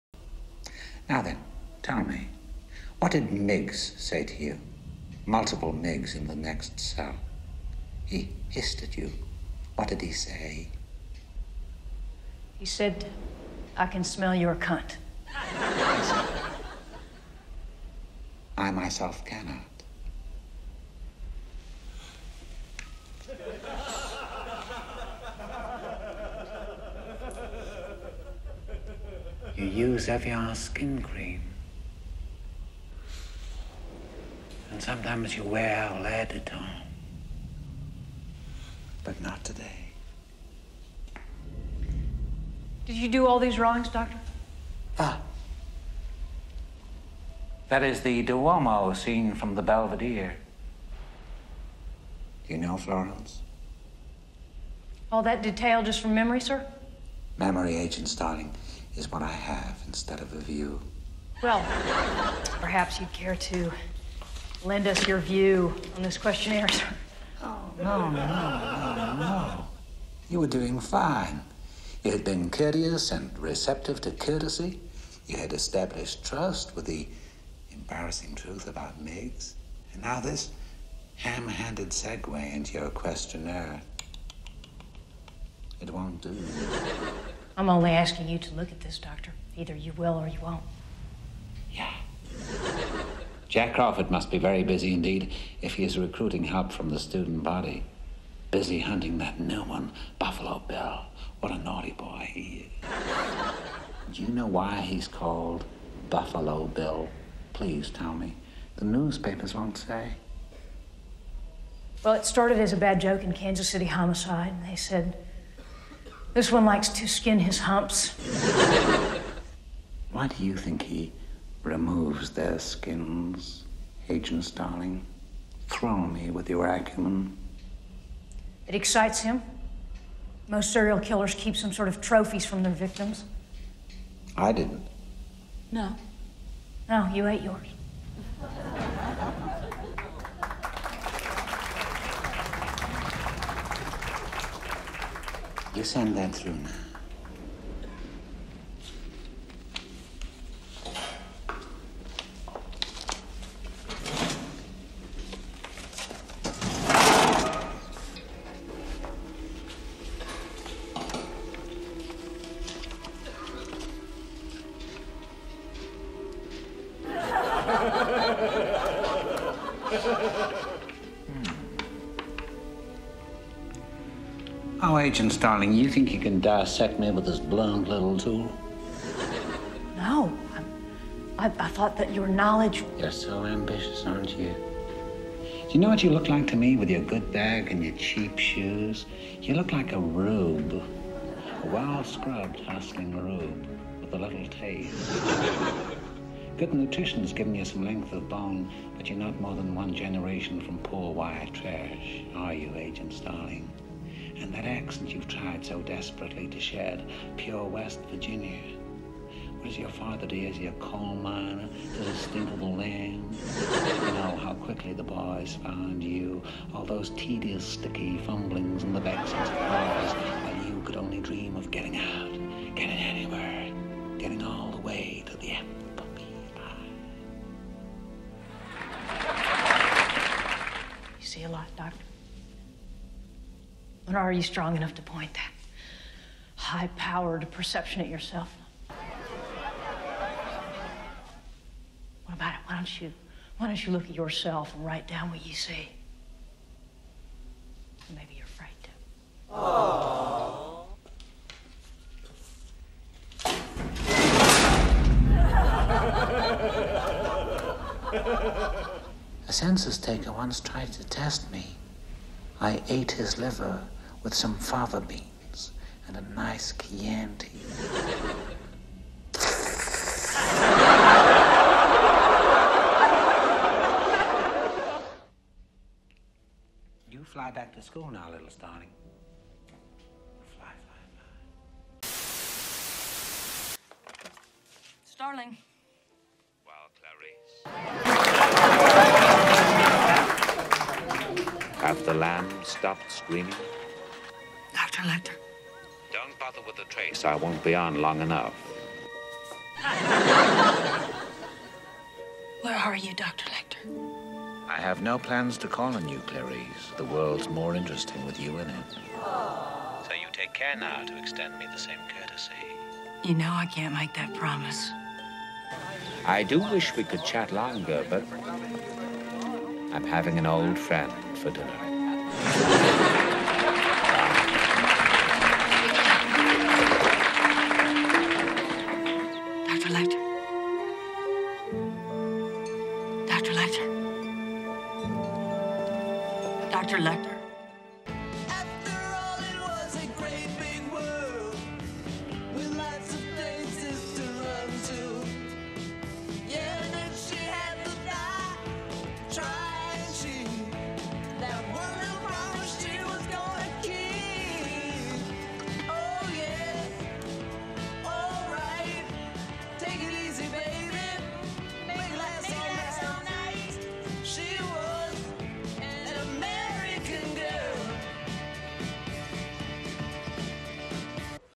Silence of the Laughs
When I first saw the movie and first heard Hannibal hiss, I could no longer take him seriously. So here you go, an attempt at self expression. (This hurts dawg.)

Ugh. This one was due for school nearly five years before. I have not been able to confront it well due to anxiety. I guess the meds finally did something as I was able to upload it.

I am still really annoyed it never got as good as I wanted, and maybe I will revise it one day, but for now, I just wanted it out of my head.